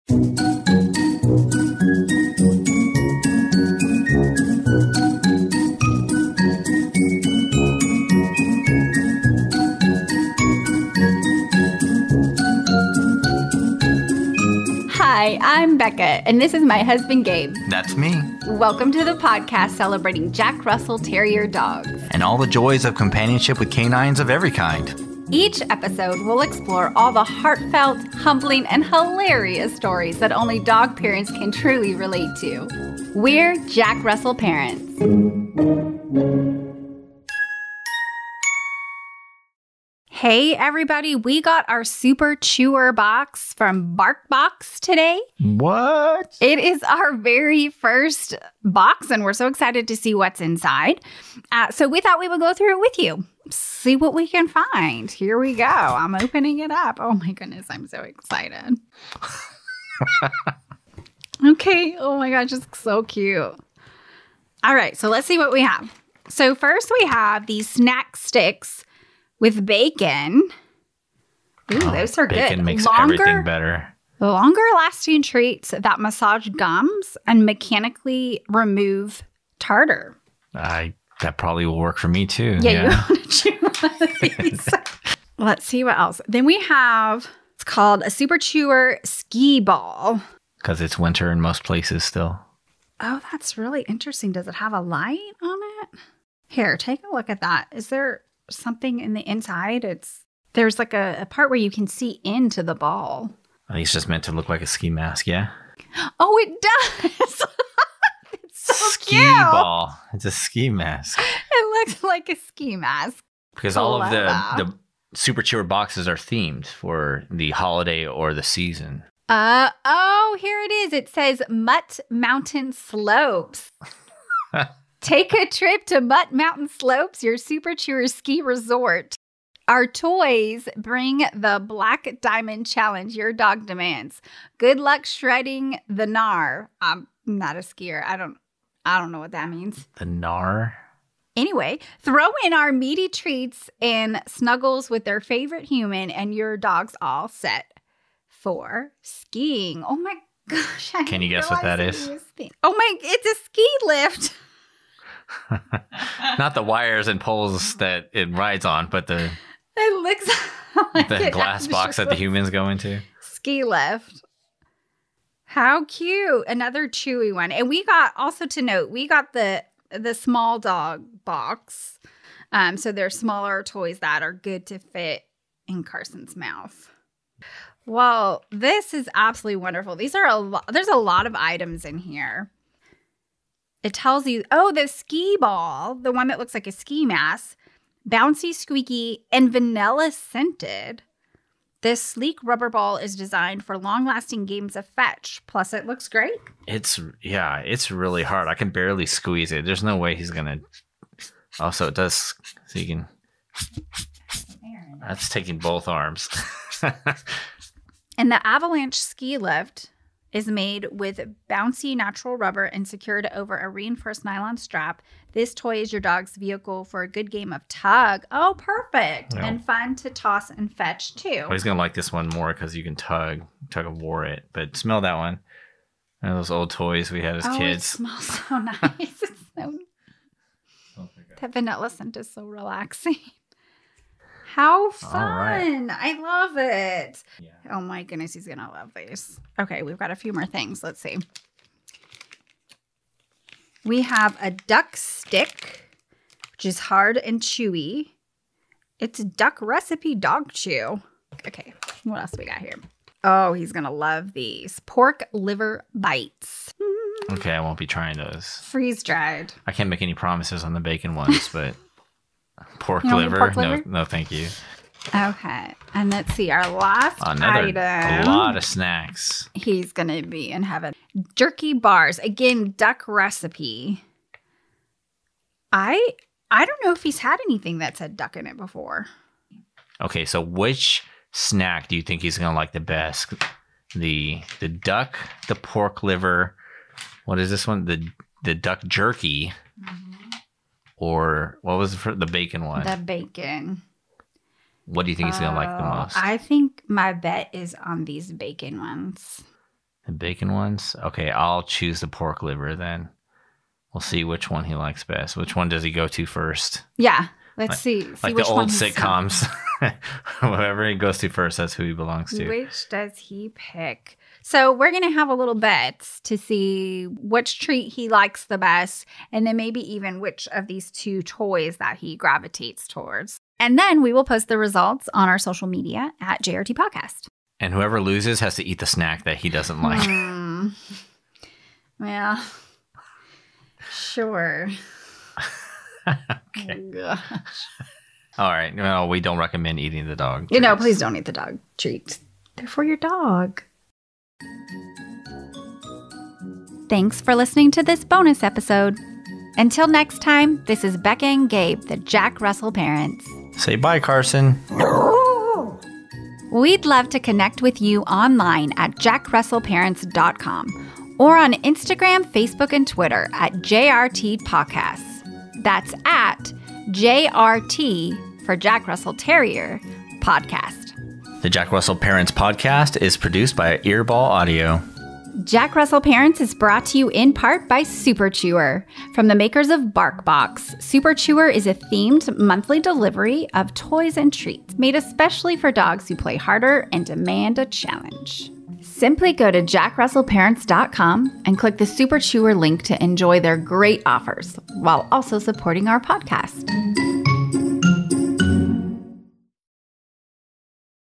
BarkBox Super Chewer Reveal
In this bonus episode, we open our first Super Chewer subscription box, check out the haul of toys and treats, and make a bet on which treat our dog Carson will like most.